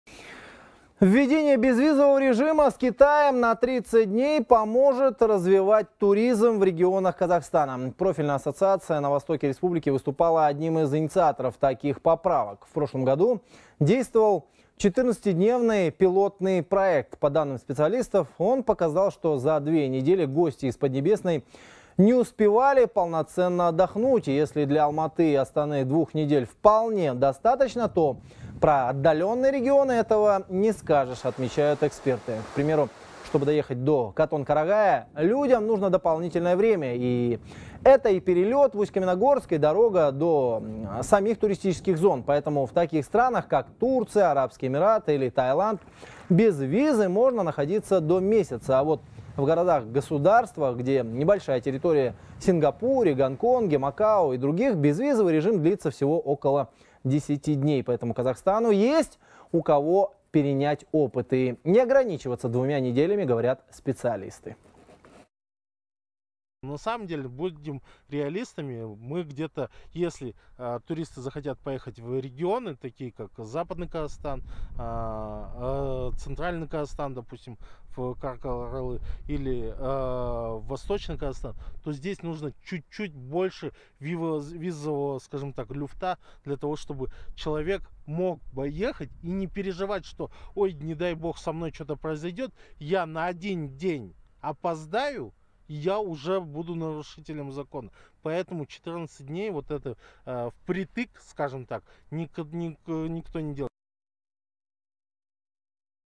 Безвизовый режим с КНР поможет развивать туризм
Введение безвизового режима с Китаем на 30 дней поможет развивать туризм в регионах Казахстана, сообщает корреспондент «Хабар 24». Профильная ассоциация на востоке республики выступала одним из инициаторов таких поправок.